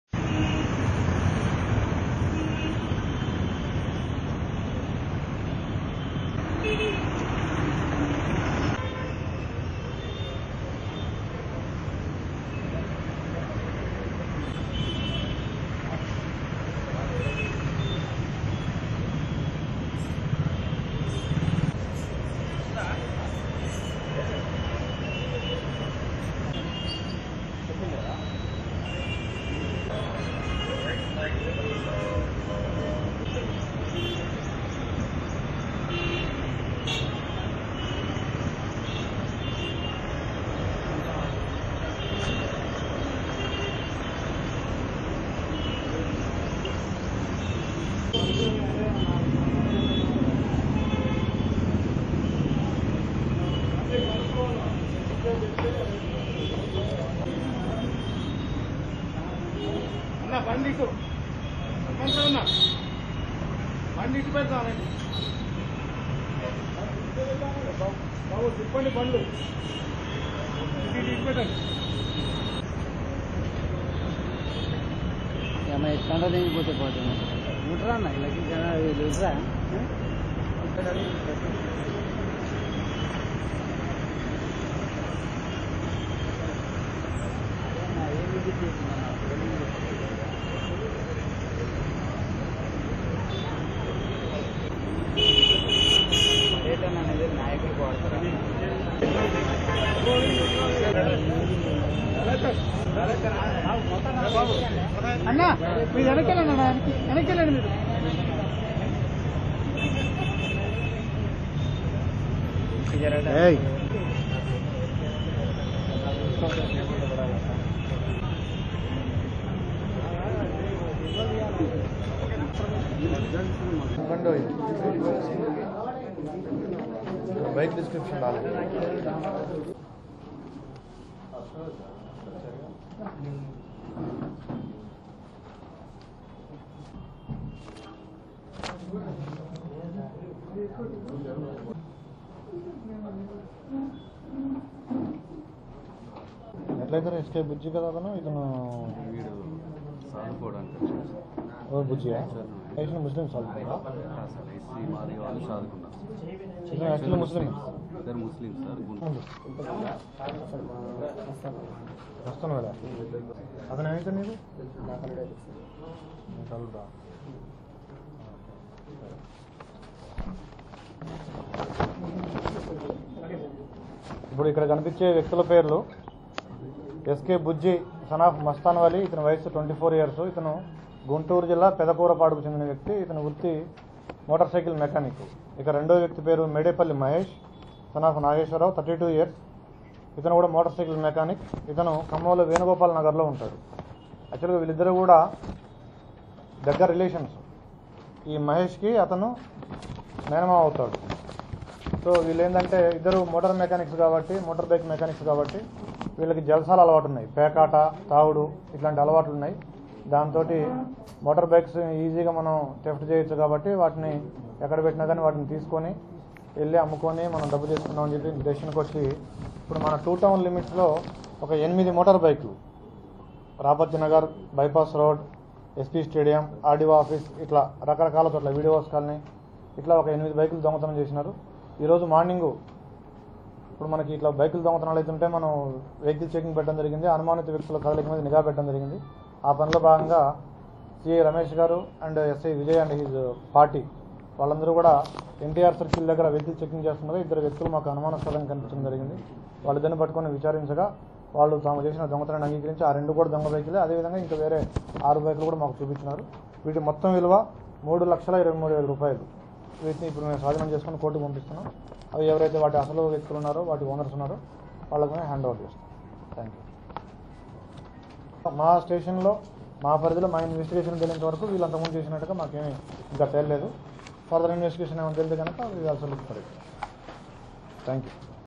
khammam tv : dsp press meet
మోటారు సైకిళ్లు దొంగతనం చేస్తున్నారన్న ఆరోపణపై ఇద్దరు వ్యక్తులను అదుపులోకి తీసుకున్నట్లు ఖమ్మం DSP సురేష్ కుమార్ తెలిపారు. వీరి నుంచి రూ.3.23 వేల విలువ గల 8 మోటారు సైకిళ్లను స్వాధీనం చేసుకున్నట్లు పేర్కొన్నారు.
అదుపులోకి తీసుకున్న వ్యక్తులనూ, స్వాధీనం చేసుకున్న వాహనాలను ఖమ్మం రెండో పట్టణ పోలీస్ ఠాణా ప్రాంగణంలో మీడియా ఎదుట ప్రవేశ పెట్టారు. అనంతరం ఏర్పాటు చేసిన సమావేశంలో కేసుకు సంబందించిన వివరాలను డీఎస్పీ వెల్లడించారు.
గుంటూరు జిల్లా పెద కూరపాడుకు చెందిన షేక్ బుజ్జీ, ఖమ్మం వేణుగోపాల్ నగర్ కు చెందిన మేడపల్లి మహేష్ ఖమ్మం వైరా రోడ్డులో బైక్ మెకానిక్ దుకాణం నడుపుతున్నారు. వీరు జల్సాలకు అలవాటు పడి చిల్లర దొంగతనాలు చేస్తున్నారు.
ఖమ్మం బైపాస్ మార్గంలో నాకాబందీ నిర్వహిస్తున్న పోలీసులకు వీరు తారస పడ్డారు. అనుమానం వచ్చిన పోలీసులు వీరిని అదుపులోకి తీసుకుని విచారణ చేశారు.
విచారణలో తామే ఈ దొంగతనాలకు పాల్పడినట్లు వారు అంగీకరించారని డీఎస్పీ ప్రకటించారు.
సమావేశంలో రెండో పట్టణ ఠాణ మడద రమేష్, ఎస్ఐ దేశిని విజయ్ కుమార్, ఐడీ దళం పోలీసులు హన్మంతరావు, లతీఫ్, రాములు తదితరులు పాల్గొన్నారు.